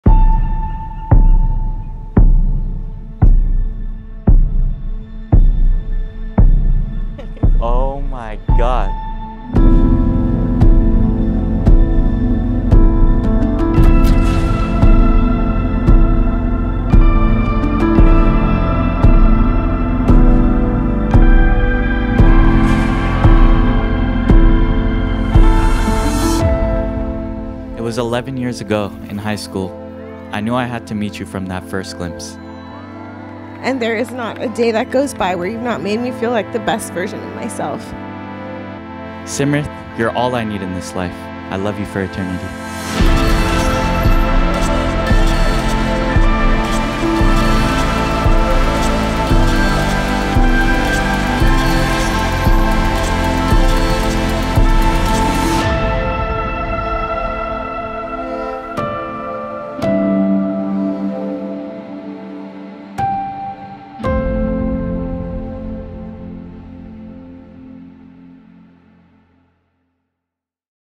Fairytale Wedding at Villa Montalvo - A Multicultural Indian Wedding Trailer by Aperina Studios
A trailer to the fairytale wedding of Kayvon & Simrath. More to come as we captured a ton more AMAZING footage.

Enjoy this Villa Montalvo Wedding.

// Gear Used //
Canon R5
Canon RF series Lenses (Canon RF 70-200mm f2.8 L IS, Canon RF 50mm f1.2L USM, Canon EF 24mm f/1.4L II, Canon RF 24-70mm f2.8 L IS, Canon RF 15-35mm f2.8L IS)
ProAm USA Crane (Jib)